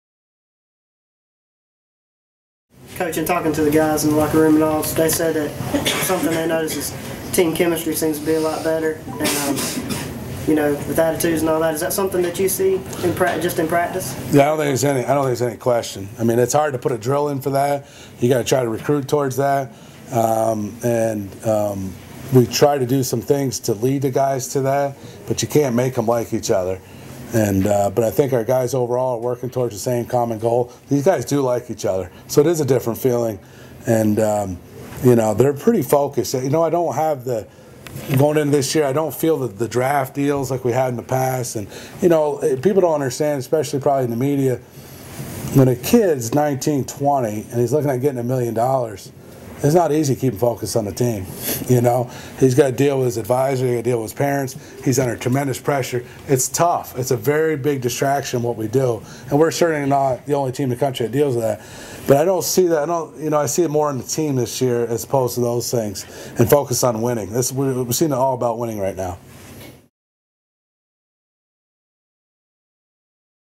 Vol baseball: Players more focused on team this year
Todd Raleigh, head coach for the University of Tennessee baseball program, talks about his team's chemistry this year versus past seasons.